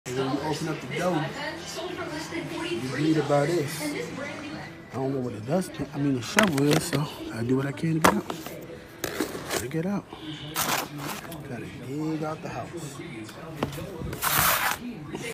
Buffalo resident shovels out house with dustpan
More than 4 feet of snow fell in Buffalo, NewYork, over the holiday weekend, leading officials to enforce a driving ban in parts of the city as crews clear roads.